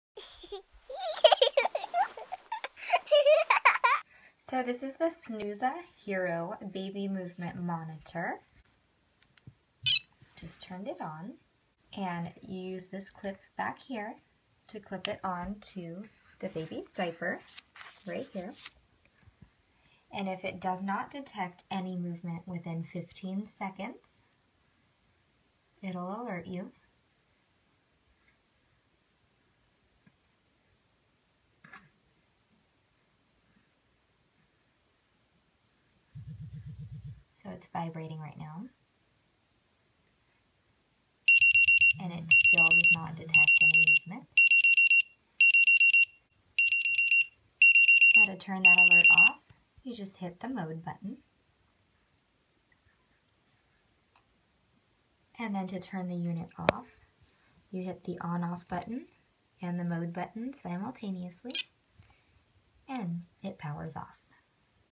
Snuza Hero Baby Movement Monitor Demo
Snuza Hero is portable and easy-to-use movement monitor which clips onto baby's diaper to ensure that normal movement is maintained. Hero detects even the slightest abdominal movement and will alert you if your baby's movements are very weak or fall to less than 8 movements per minute. If no abdominal movement is detected at all for a period of 15 seconds, Hero will vibrate gently. Often this vibration is enough to rouse the baby, and Hero will revert to monitoring mode. After three vibration/rouse incidents, the Rouse Warning will alert you to the fact that your baby's abdominal movements have stopped for 15 seconds on three occasions. If no further movement is detected for another 5 seconds, an alarm will sound to alert you. The Snuza Hero will pick up external movement which makes it not suitable for co-sleeping babies or bedsharing.